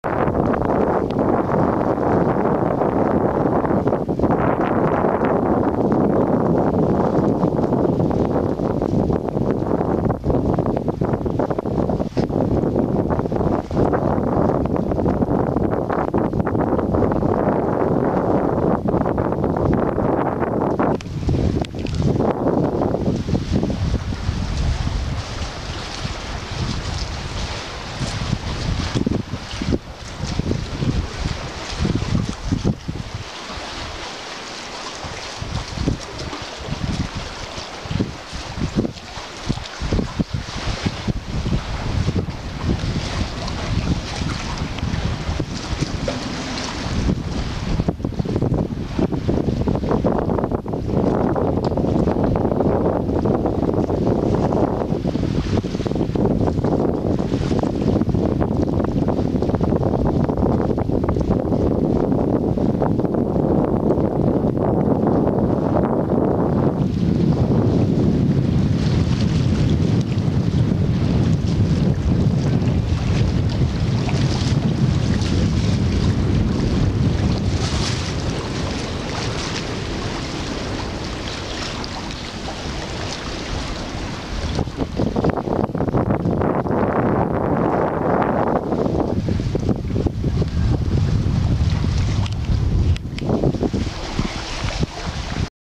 Graal-Müritz Seebrücke Rundblick 2015
Seebrücke Graal-Müritz

Die 350 m lange Seebrücke in Graal-Müritz ist ein Anziehungspunkt für jeden Besucher. Von hier aus haben Sie einen einmaligen Blick auf das Ostseeheilbad und auch auf die weite Ostsee.